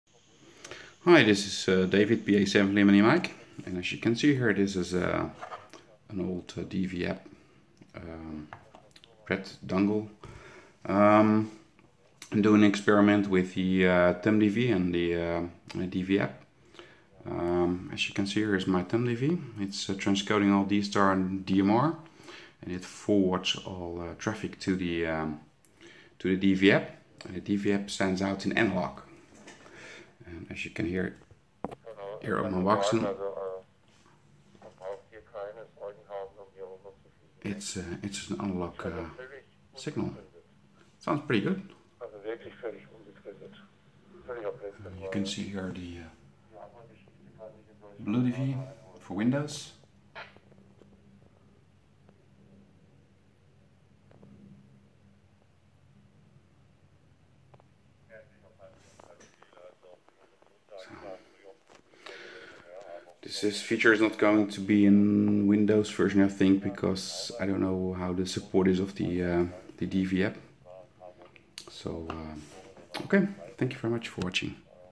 Experimenting with BlueDV for Windows with ThumbDV, DVAP and an analog radio's ( 18-mar-2018 )
The ThumbDV is transcoding the DSTAR/DMR voice to the DVAP. The DVAP sends out in Analog mode. Sound is very nice!!
Do not know if I add this to the Windows version. Do not know is there is still support for the DVAP.